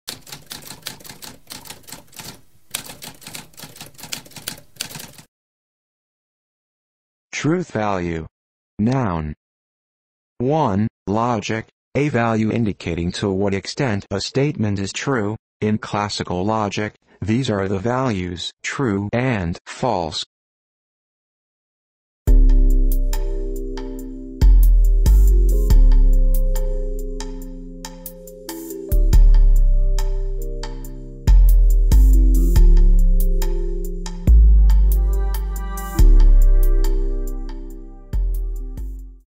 What does truth value mean?
A spoken definition of truth value.

Intro Sound:
Typewriter - Tamskp